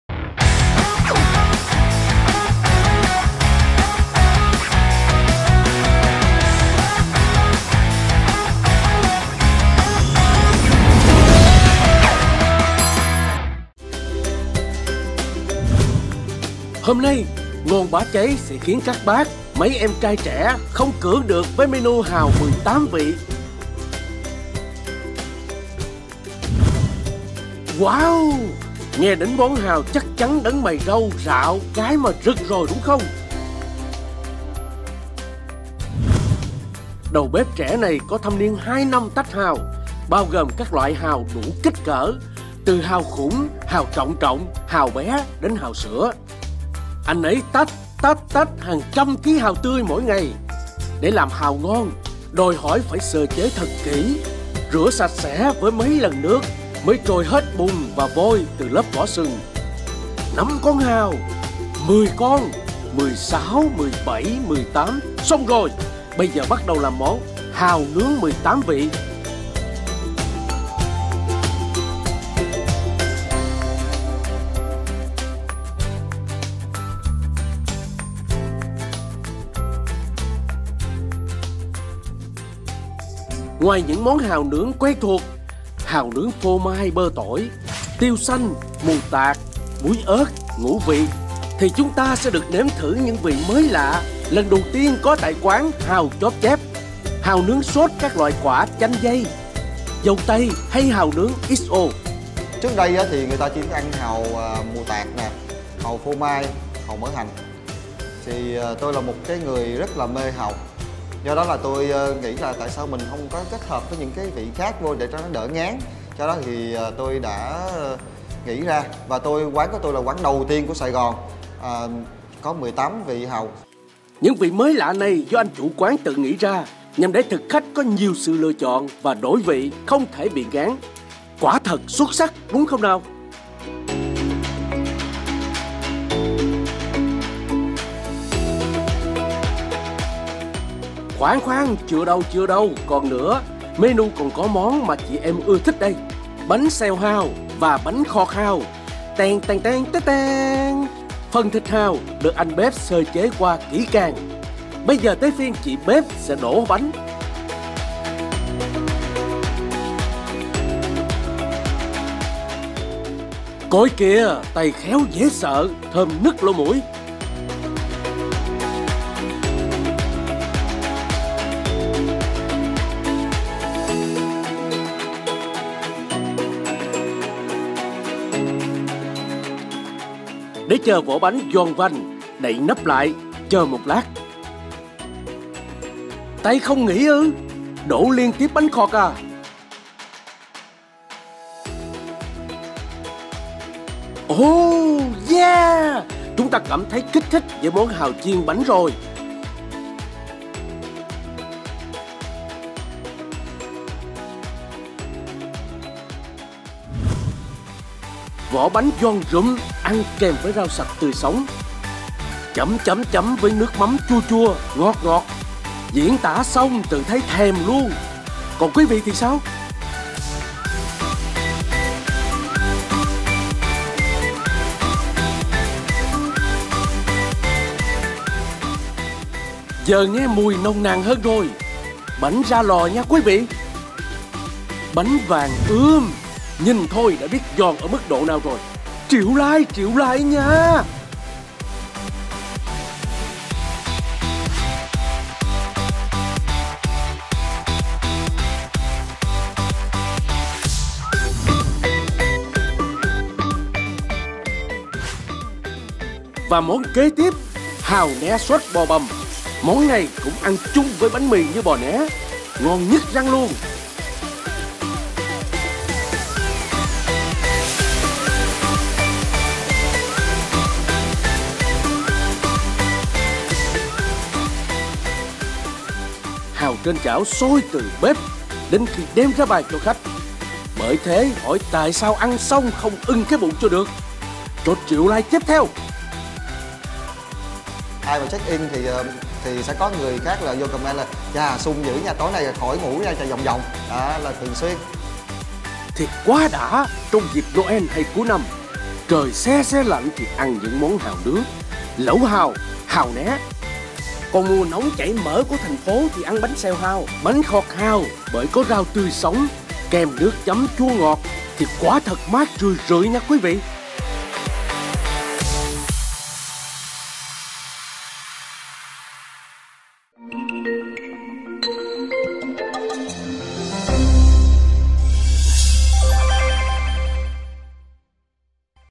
Hàu Chóp Chép - Thế giới hàu đầu tiên ở Sài Gòn có 18 vị - SAIGONTV
Nếu bạn là thánh cuồng hàu thì không thể bỏ qua hàu 18 vị hấp dẫn ở TP.HCM. Dâu tây, chanh dây, mù tạt, mắm nhĩ... là những hương vị độc đáo có trong mẹt hàu và còn có thêm món bánh xèo hàu, bánh khọt hàu...ngon "bá cháy".